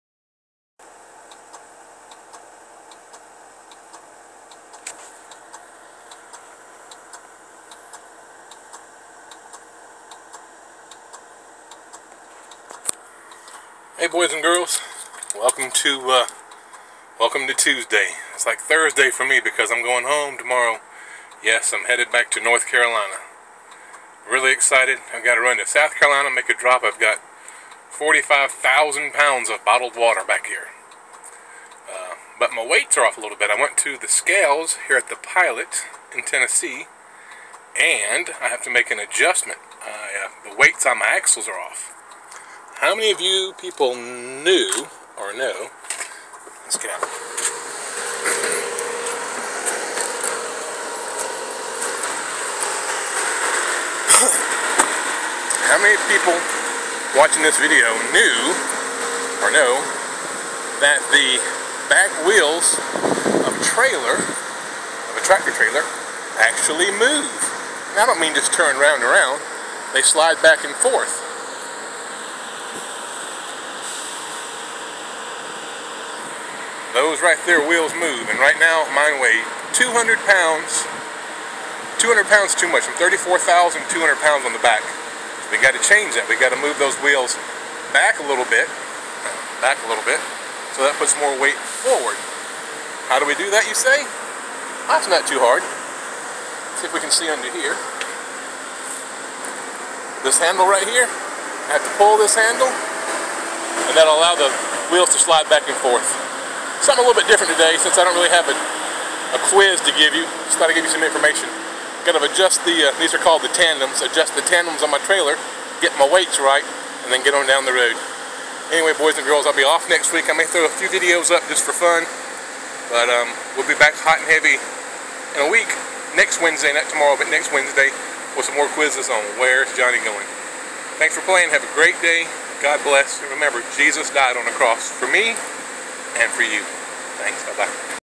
I weigh 39 Tons!!!
Adjusting the tandem axles to balance the load